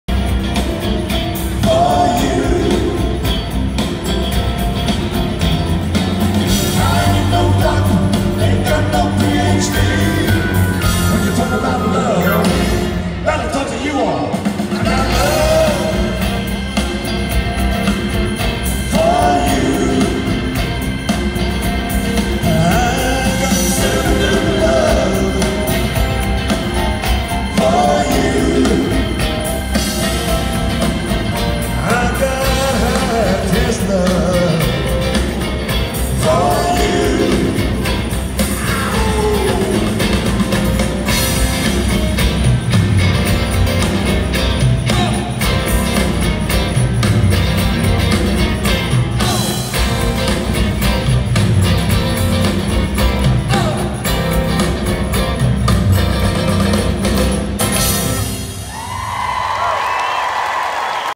Tony Cottrill FOH Live Mix Engineer
Bossier City, LA - Centurylink Center - EXILE direct support for Alabama - Tony Cottrill Front of House Live Mix Engineer - July 11, 2019